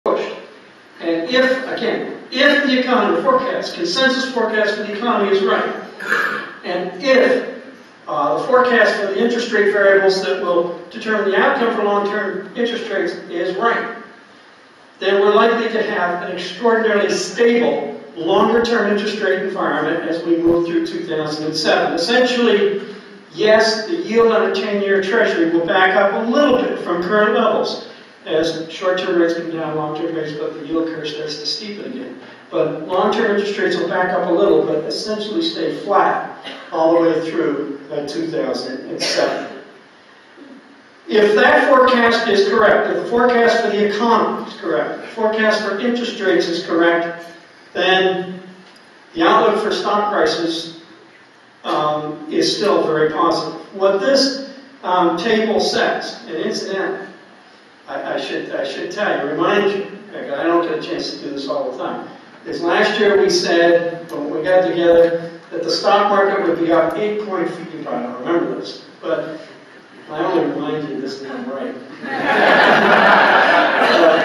Hugh Johnson
Hugh Johnson of Johnson Illington Advisors offered his annual outlook on the stock market in the coming year at the Marriott Hotel in Colonie, New York this morning Dec. 19, 2006.